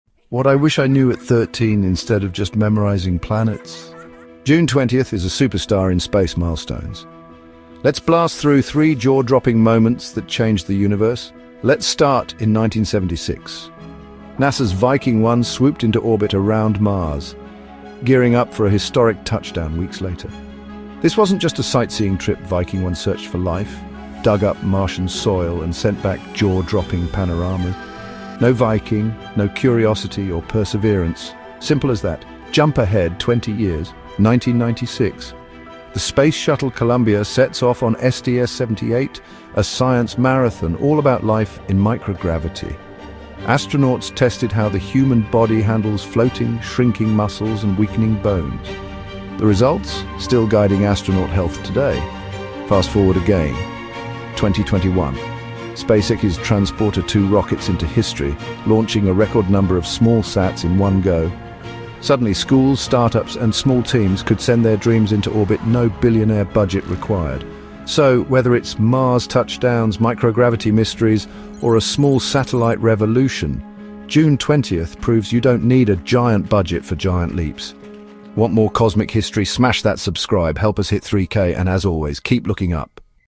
On This Day in Space: 20 June - Mars, Microgravity & Small Sats
🗓️ On June 20:

1976: Viking 1 entered Mars orbit, changing planetary science forever.

1996: Columbia STS-78 launched to study life sciences in space.

2021: SpaceX launched Transporter-2, a record-setting SmallSat mission.

Big discoveries, small satellites, and one amazing day in space history.